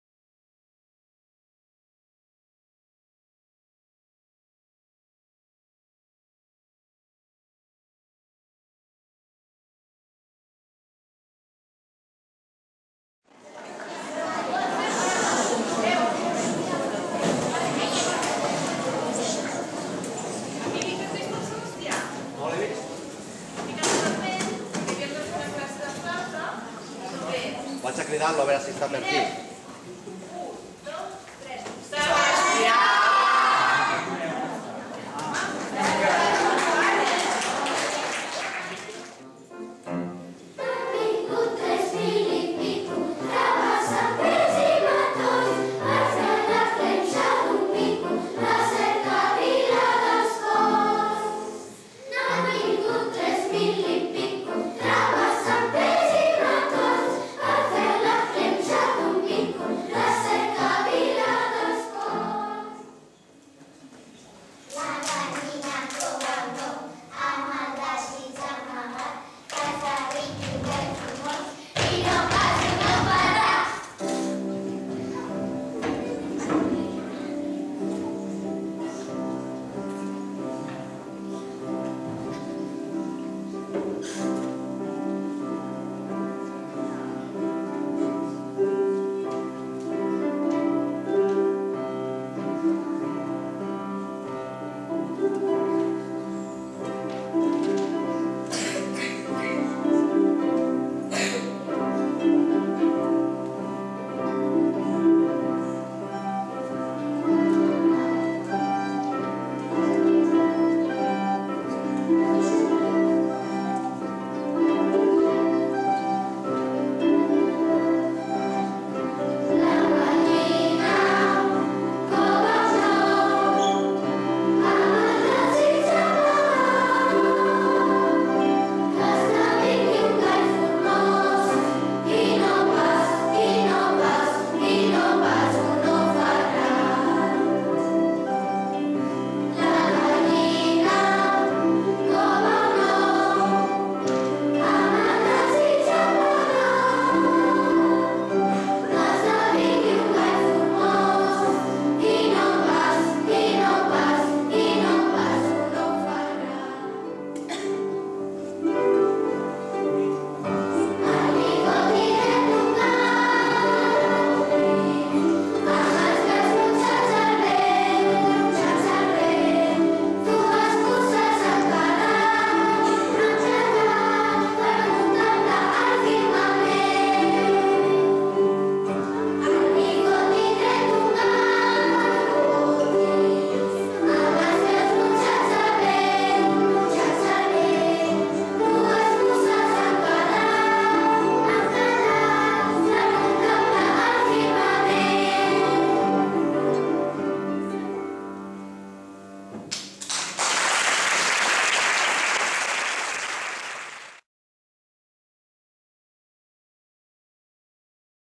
Sant Jordi 2017: Bestiari salvatge
El passat dia 18 d’abril a la Sala Petita de l’Escola, tot just passat Setmana Santa, vàrem fer la presentació del llibre: “Bestiari salvatge d’estar per casa” de Sebastià Serra. L’acte de presentació va comptar amb la participació dels alumnes de cant coral d’elemental sota la direcció d’Isabel Pla i dels conjunts de nivell elemental de flauta de bec i arpa.